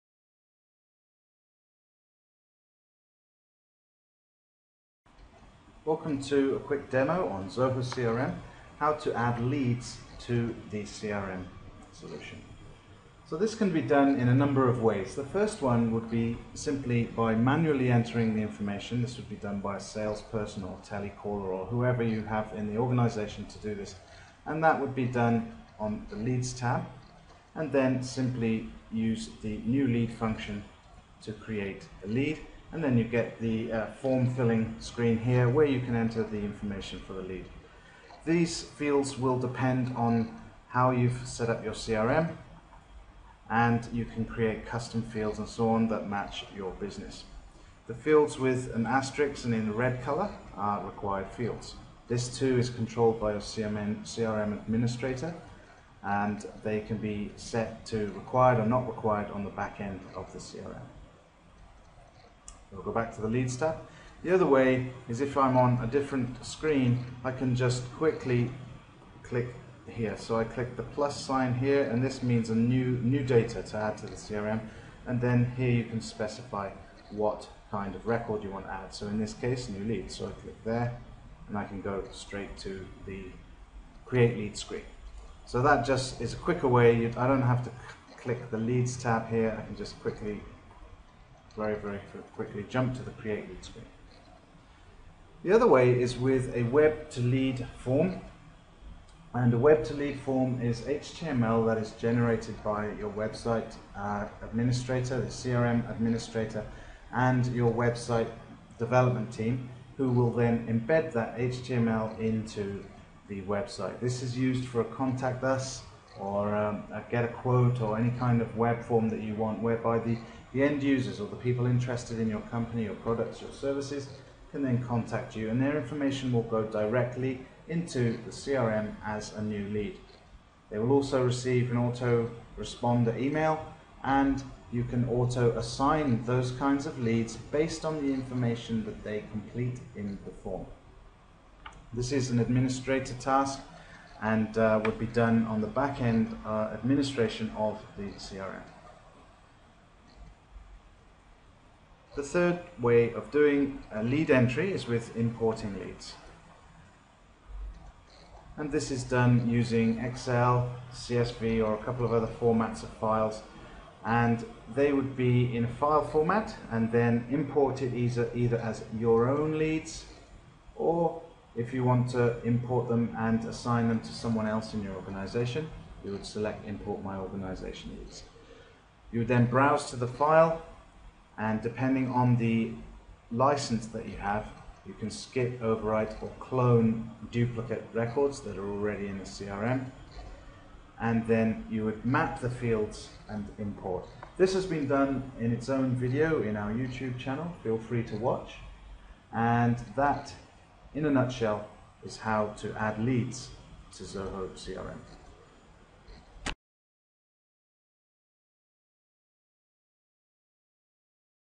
Adding Leads in Zoho CRM
There are a few ways of adding leads in Zoho CRM, here's a demo of them!